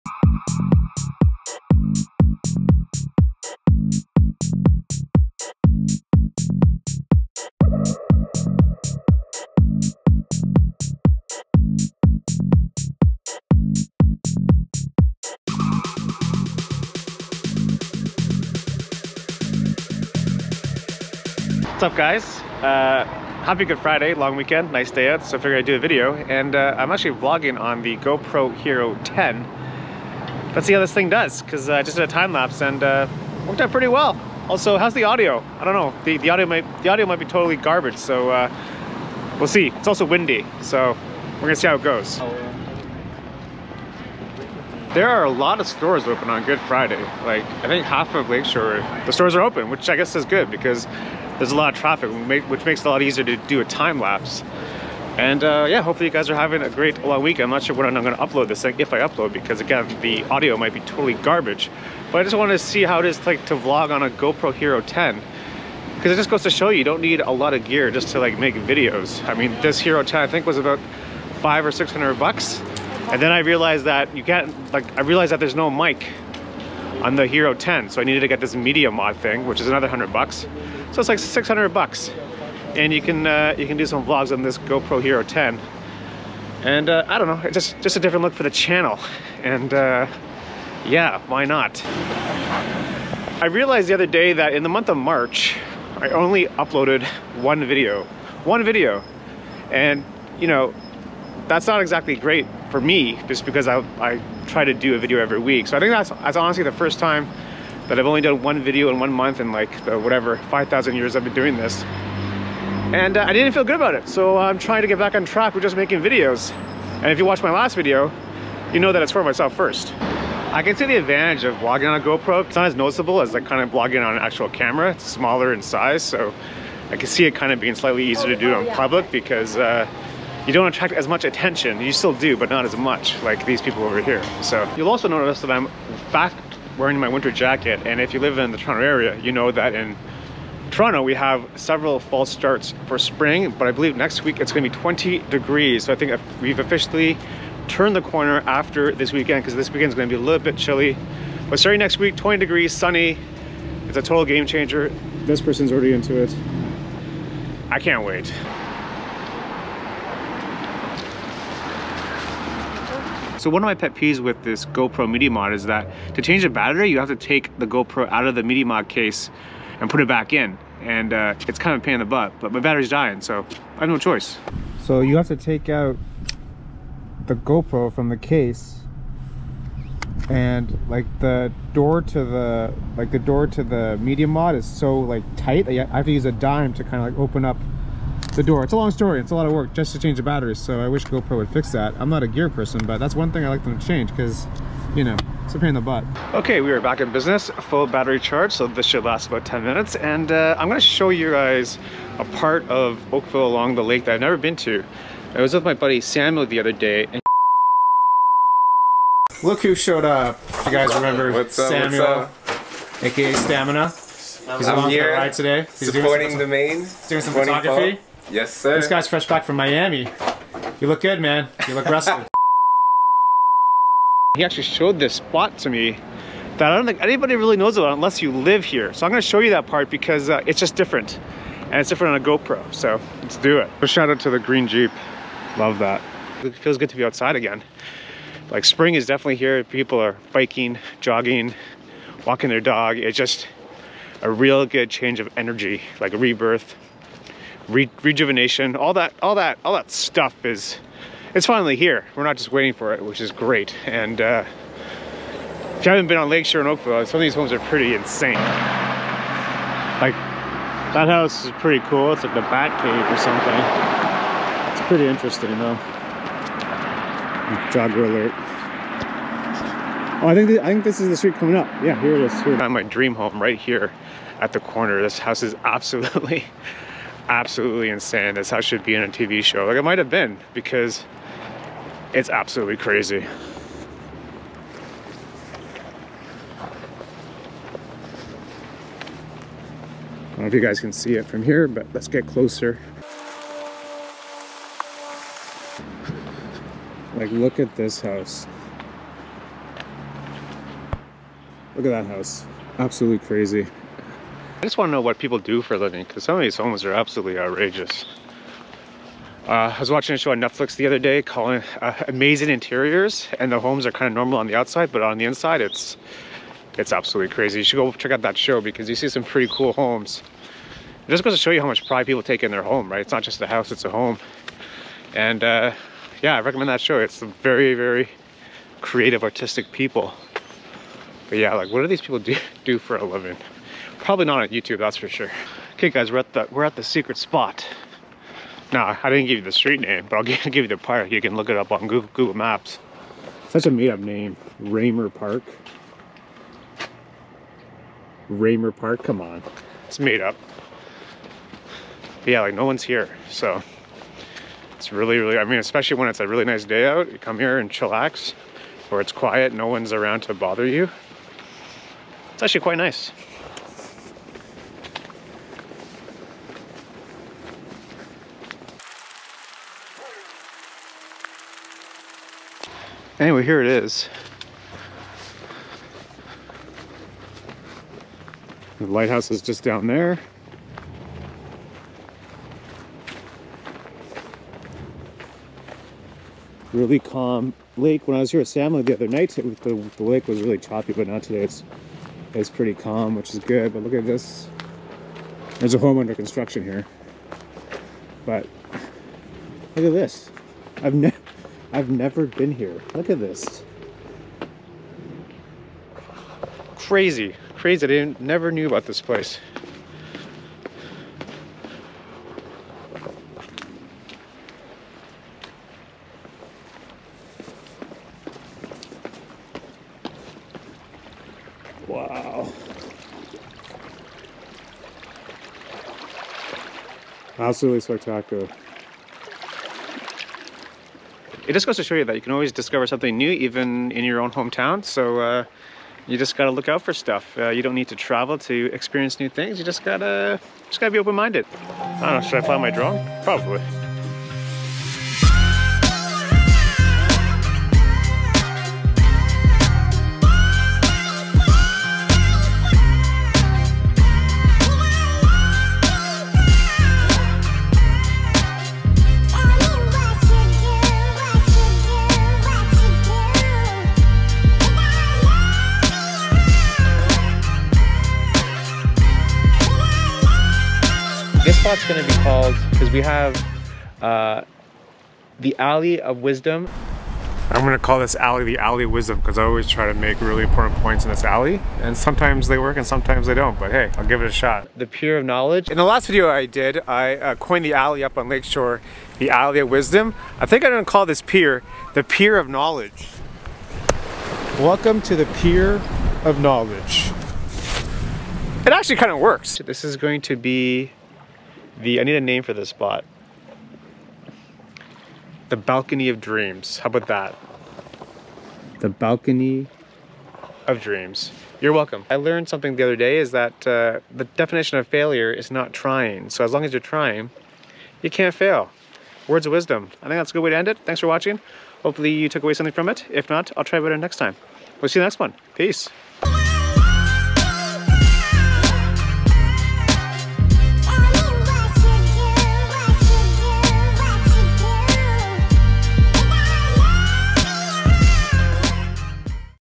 The Definition of Failure.🤘😬
I noticed that in March I only uploaded 1 video which is unusual for me so I'm back to making weekly vids (I hope).

I recently acquired the GoPro Hero 10 Black and figured I would vlog on it to see how it handles in the real world. I was pleasantly surprised at how well the audio was, despite the winds, and the footage was super smooth.

I also show you a spot in Oakville that I had no idea existed until my friend Samuel showed me. It's a pretty cool spot and one that I intend to make use of.

It's not a vlog until I share some words of wisdom with respect to what failure actually is. It's normal to fear failure, probably even more so with respect to success, so hopefully I help motivate a little here.